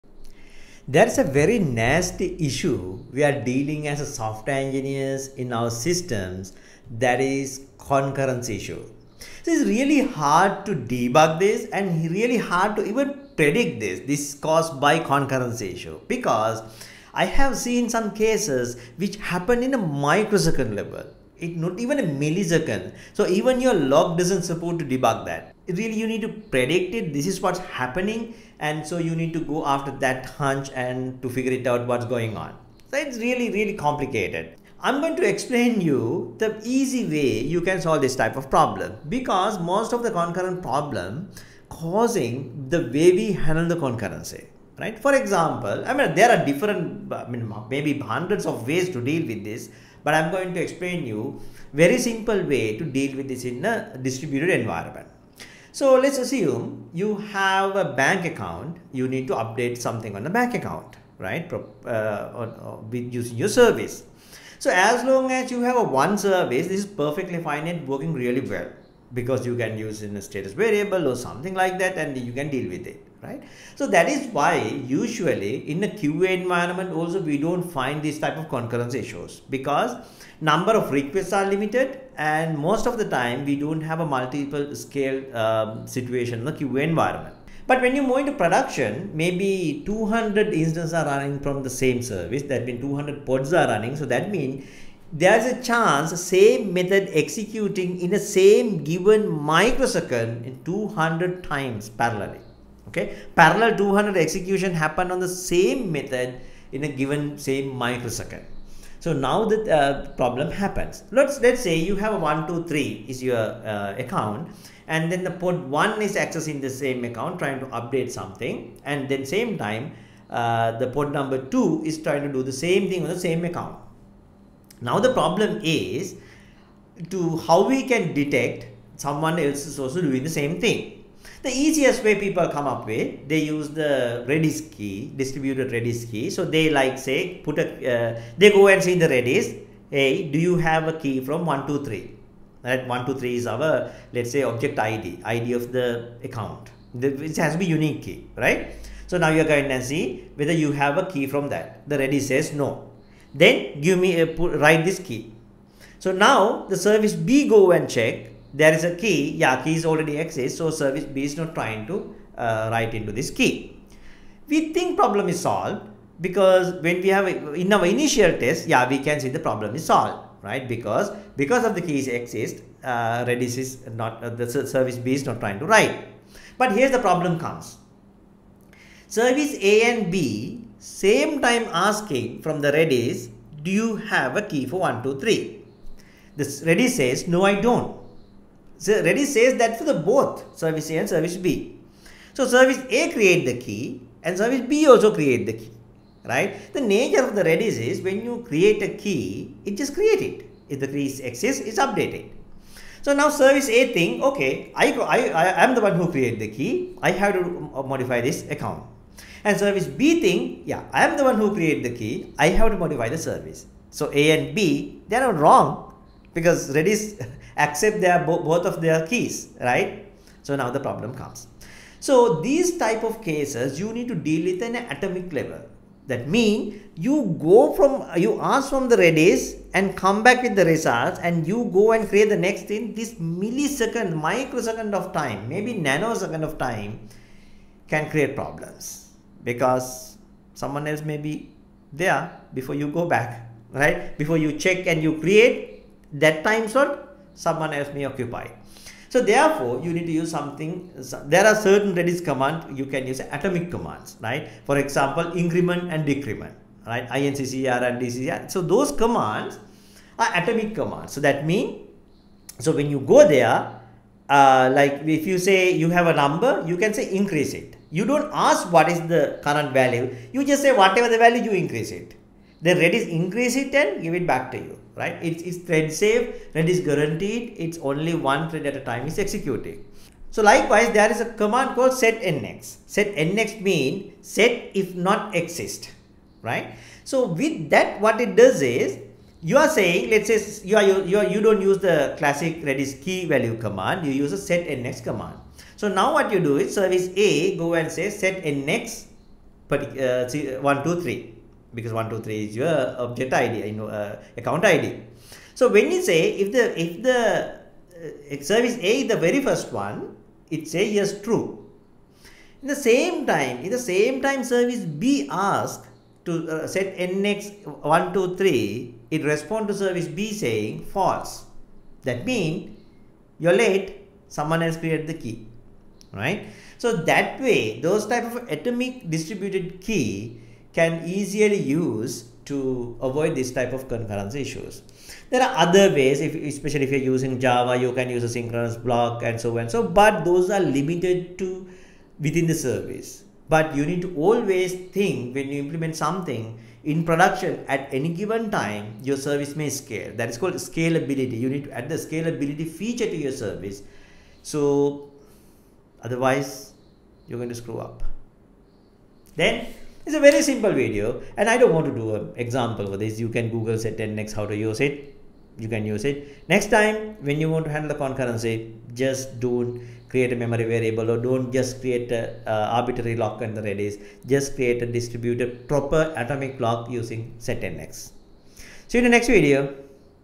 Distributed concurrency control
Today, we dive deep into the fascinating world of distributed concurrency control in microservices. Microservices architecture brings many advantages, but managing concurrent access and data consistency across distributed systems poses significant challenges. We’ll explore the key concepts, strategies, and best practices to effectively handle distributed concurrency, ensuring data integrity and system reliability.

Whether you're a developer, architect, or tech enthusiast, this video will provide you with valuable insights and practical knowledge to enhance your understanding and implementation of concurrency control in distributed microservices.